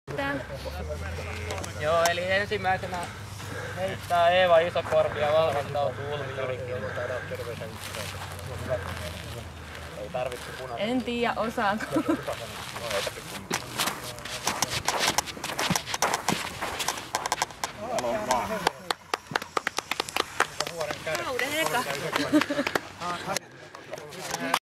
Bootthrowing by Eeva Isokorpi in South-Finnish Spring Cup 1 2014
South-Finnish Spring Cup 1 was organized in Riihimäki on the 11th May 2014. (Filmed by Jari Isokorpi)